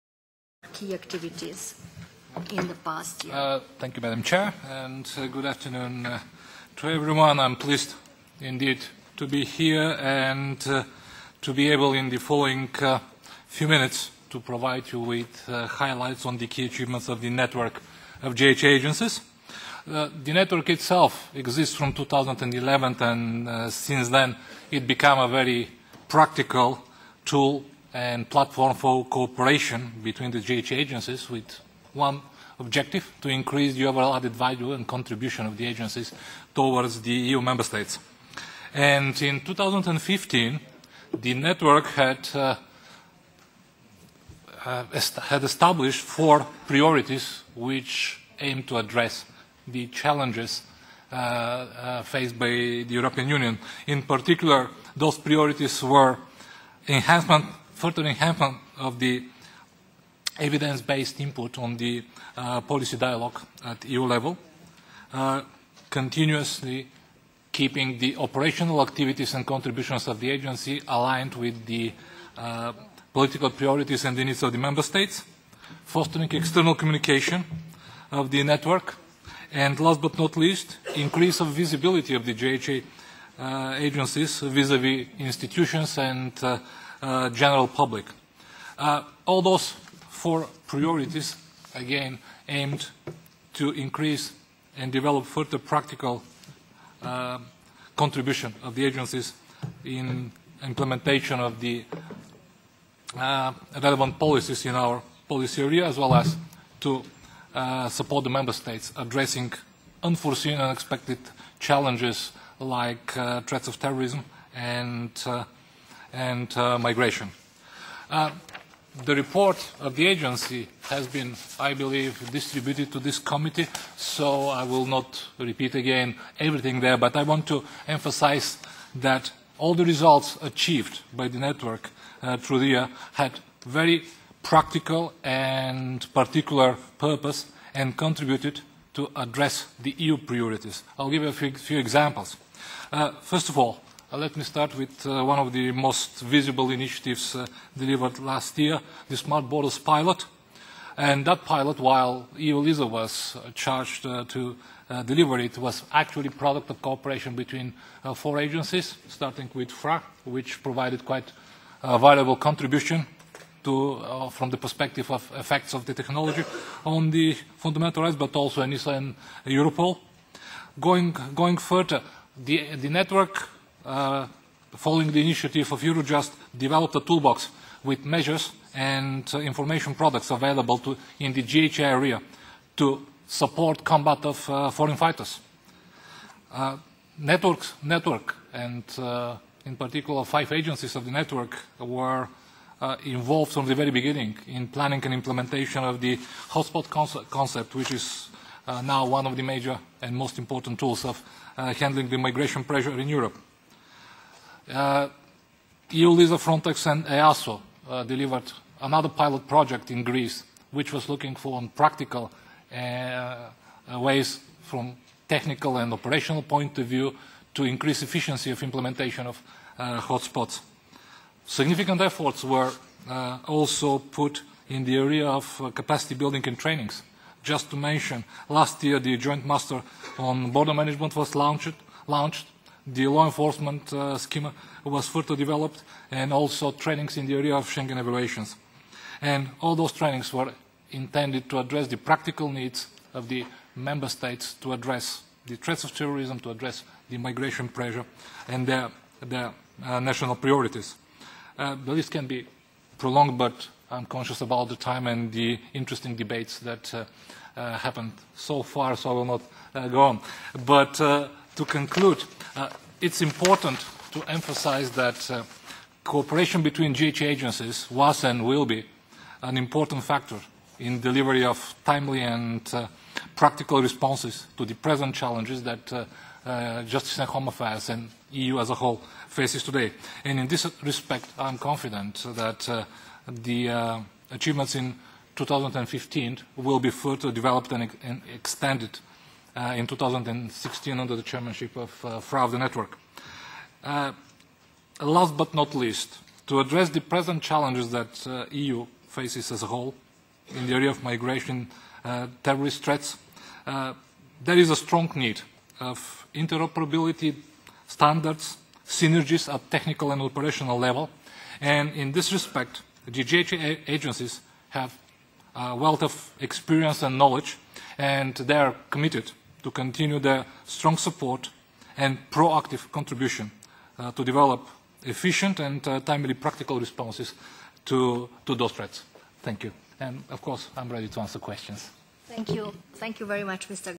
Krum Garkov at LIBE Committee 25-01-2016
eu-LISA Executive Director Krum Garkov presenting the results of the JHAA Network's activities in 2015 (under eu-LISA's chairmanship) to the LIBE Committee on Monday 25th January 2016.

This video clip is an extract drawn from the European Parliament's official web-stream.